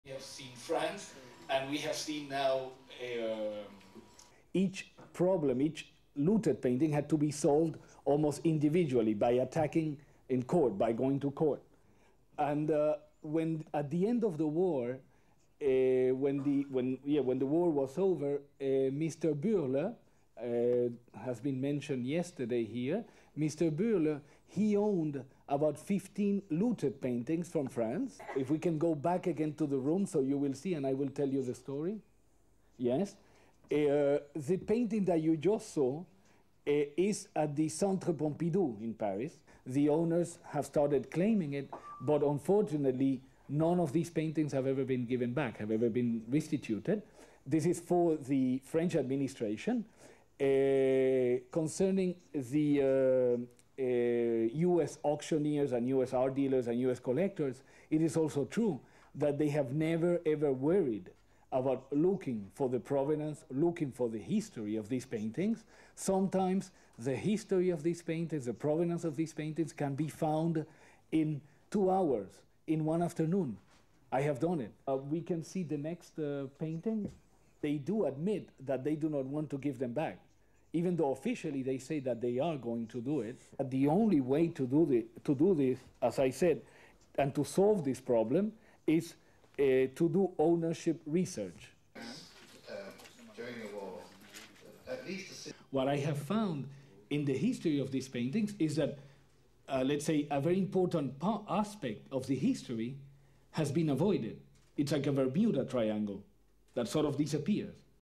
Switzerland -Dispute over looted art
(25 Jun 1997) T/I:   10:20:15
A conference in Geneva, Switzerland, discussed on Wednesday (25/6) the contentious issue of art works and other treasures looted by the Nazis.
The conference - "Property and Restitution - a moral responsibility to history" - was organised by the Simon Wiesenthal Centre. Participants discussed recent reports that Switzerland allegedly profited from gold and treasures that rightly belonged to Jews killed in the holocaust.
And it concentrated on the fate of art works seized by the Nazis.
SHOWS:
GENEVA, SWITZERLAND. 25/06.
(English commentary from Hector Feliciano runs throughout)
WS conference;
MS slide on projector;
CU letter on projector screen, cataloguing paintings;
CU paintings on slides seen on projector screen;
SOT, Hector Feliciano, author of "Le Musee Perdu" (English), saying that owners have started claiming for their seized paintings, but that none have been returned, and that museums don't often look for the provenances of the paintings, even though it is an easy task to do so;
VS stills of paintings from slides on projector screen;
SOT continues, Feliciano, saying that collectors don't want to return the paintings to the original Jewish owners;
WS conference;
SOT continues, Feliciano, saying that the paintings' histories are like a Bermuda Triangle, in which they just disappear.
1.48